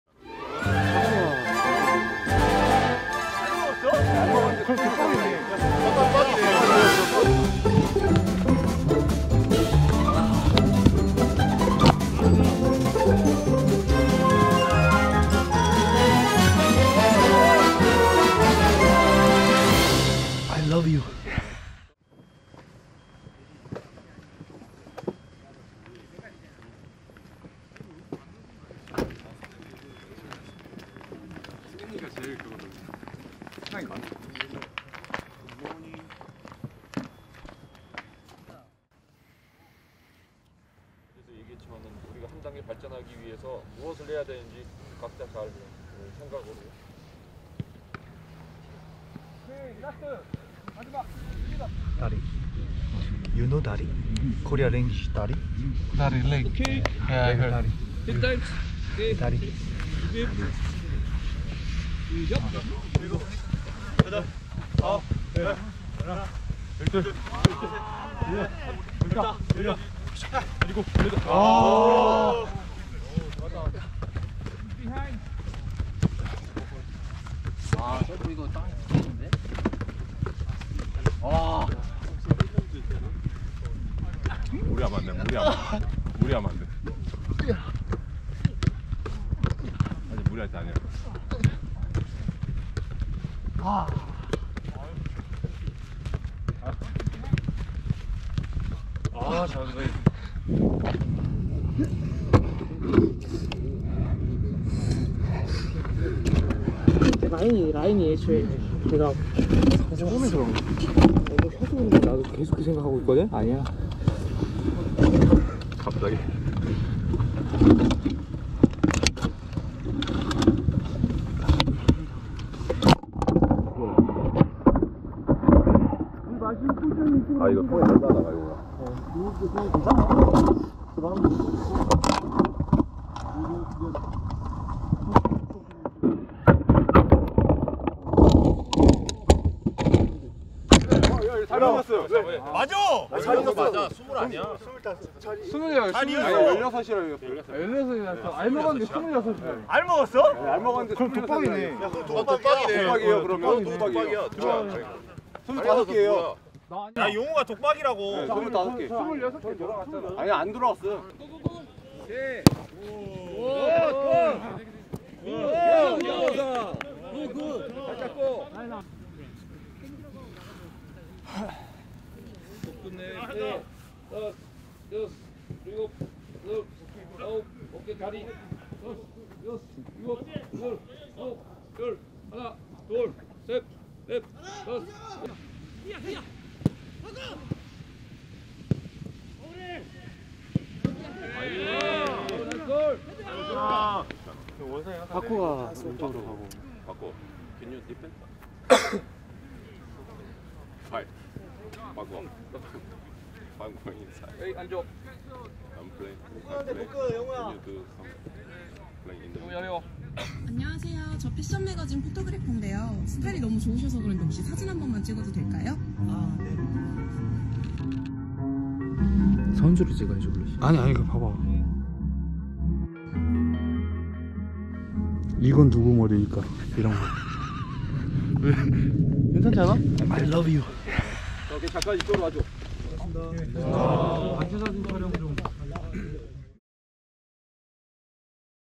완전체가 된 울산의 겨울 전지훈련 밀착 로그! [UH•Log]
[UH•Log]

완전체가 된 호랑이들의 열정으로

따뜻하게 데워진

울산 겨울 전지훈련 훈련 밀착 로그!🐯

00:22 출근길
00:42 윤구,영우,민혁 반가워💙
00:52 다리에게 다리아냐고 물어보는 정캡
01:03 울 민혁이 수비 열정
01:14 다치면 안 돼
01:44 수분보충 놓치지마요
02:22 다리둥절
02:41 원상이는 빨라
03:07 스웨덴 라푼젤
03:30 국가대표 출신 포토그래퍼